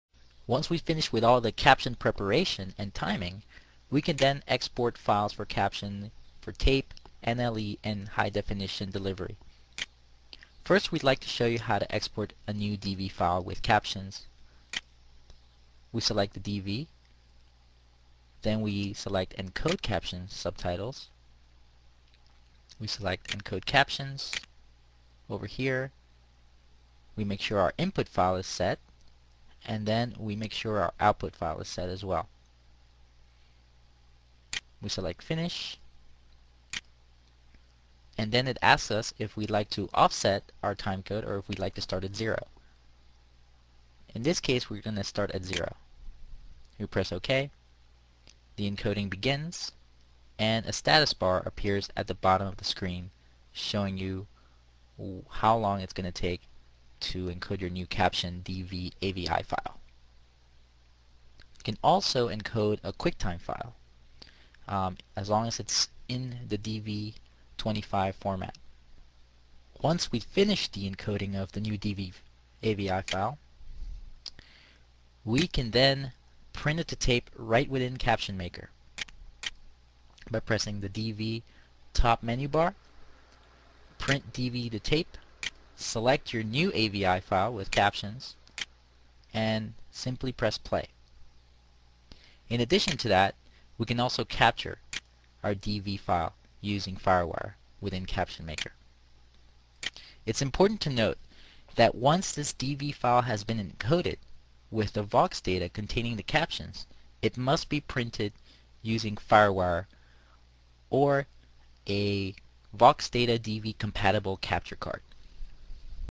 CaptionMaker - export DV 25 video file with caption data
DV 25 and DV 50 files are compatible with DVCAM, MiniDV, and DVCPRO decks that can accept a firewire connection from Mac or Windows. CaptionMaker software can add VAUX data to the DV files for closed captioning delivery.